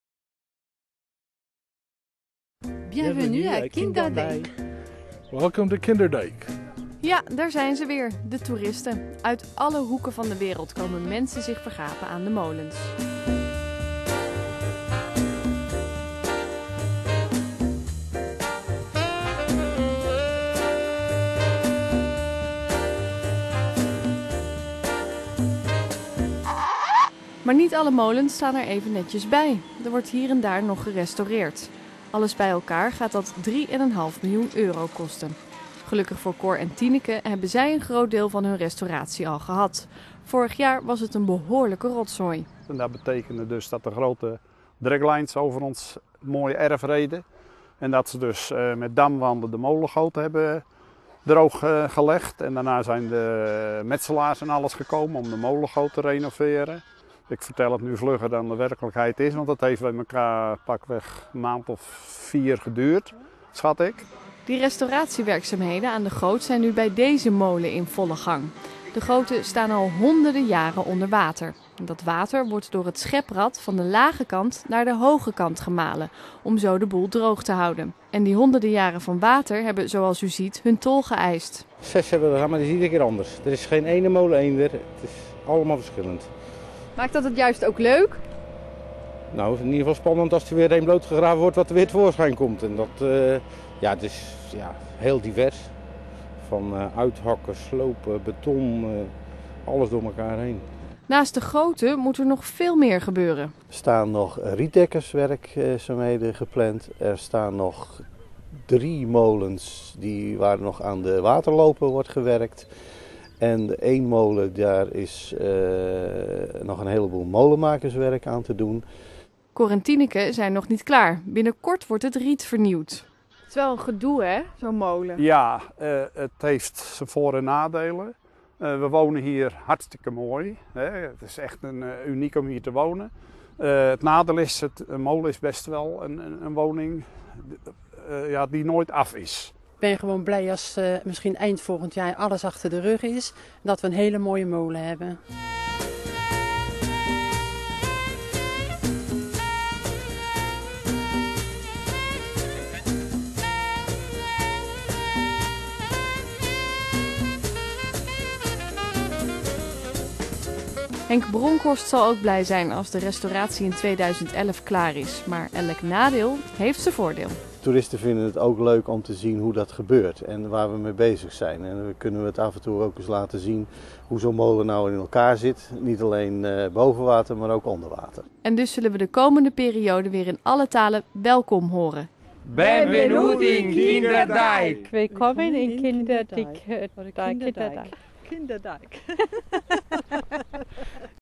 Renovatie molens Kinderdijk
Het toeristische seizoen in Kinderdijk is weer geopend. Met bussen tegelijk komen de Japanners, Fransen en Duitsers de molens bekijken. Maar is Kinderdijk wel klaar voor al die toeristen? Wie beter kijkt ziet vorkheftrucks naast molens staan en ziet metselaars hard aan het werk.